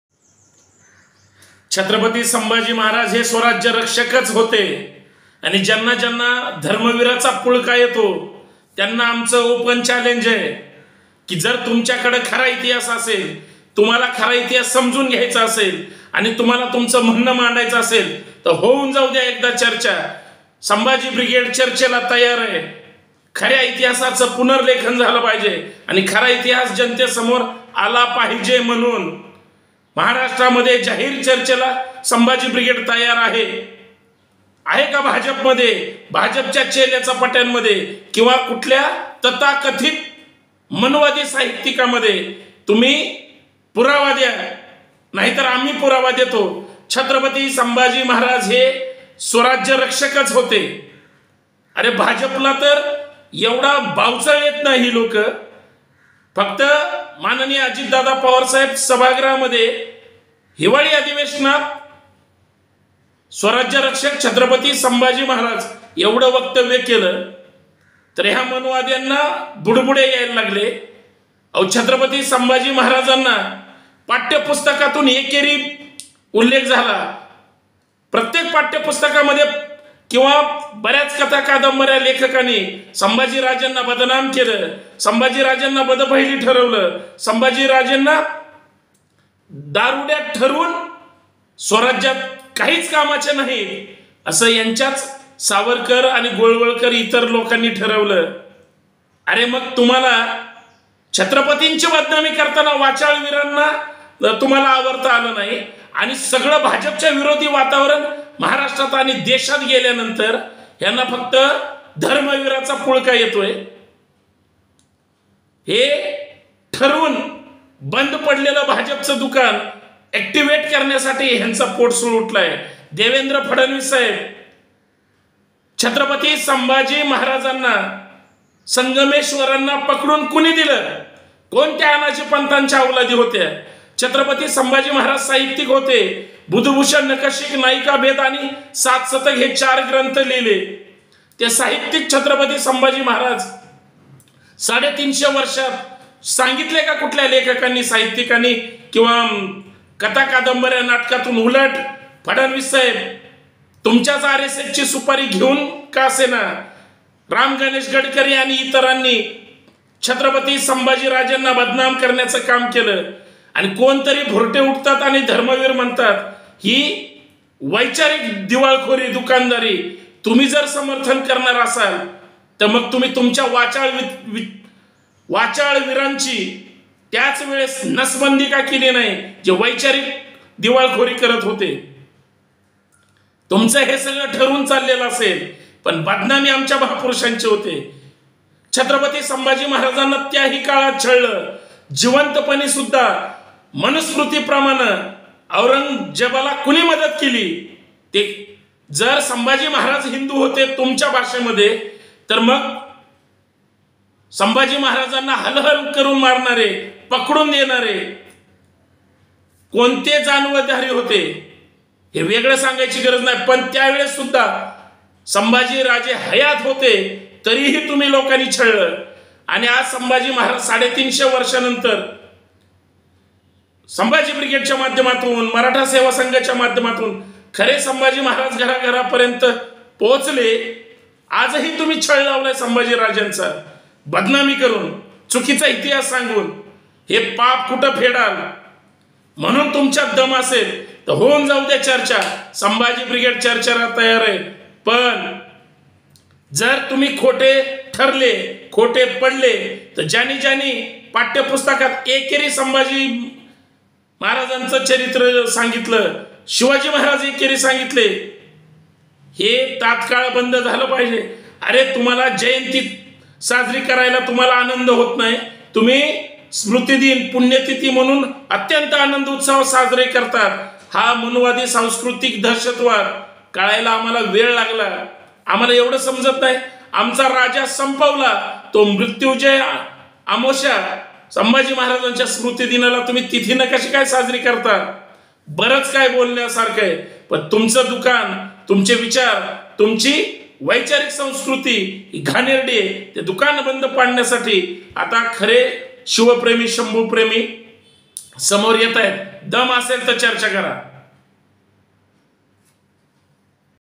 गद्दार साले, इतिहास गाडायला निघालेत... - Santosh Shinde, Sambhaji Brigade
Pune | धर्मवीर च्या नावाने भाजप खोटे बोलून तोंडघशी पडली...? - संतोष शिंदे, संभाजी ब्रिगेड